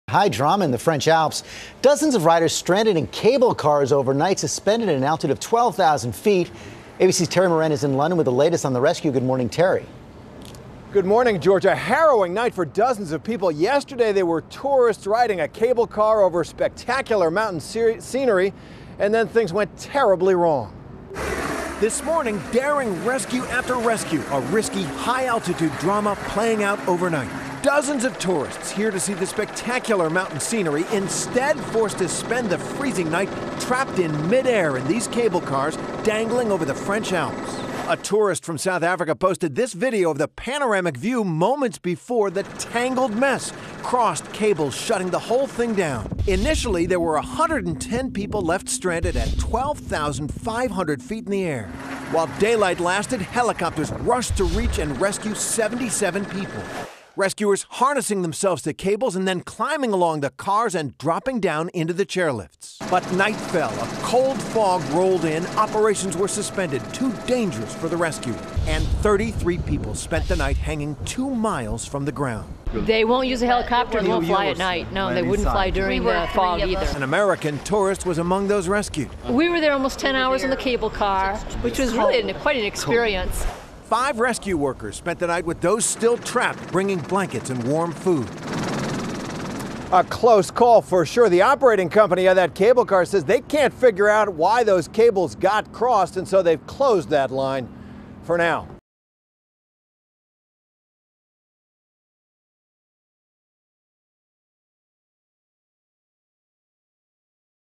Stranded Passengers Rescued From Cable Cars in the French Alps
French and Italian responders evacuated 110 people who were stranded thousands of feet above ground, after the cables got tangled.